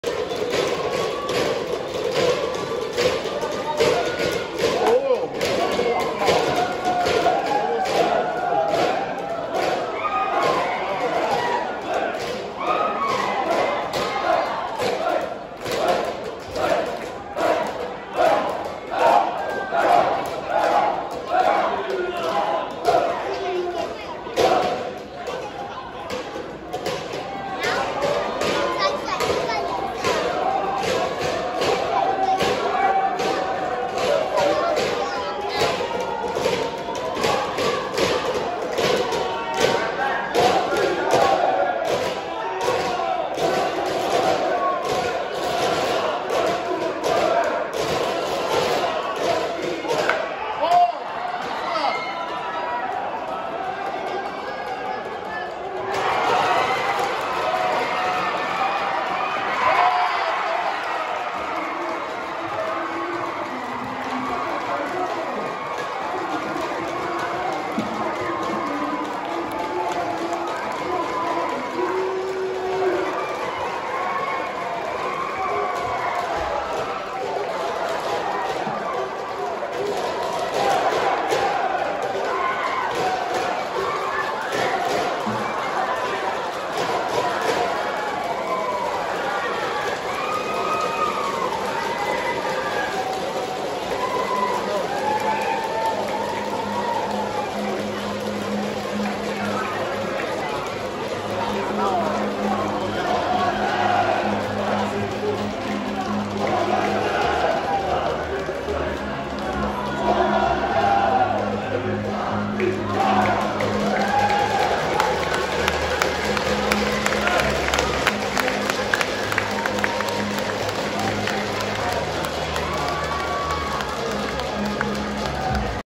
20230304 穀保家商 劉俊緯 再見高飛犧牲打(111學年度木棒聯賽冠軍戰)
打到突破僵局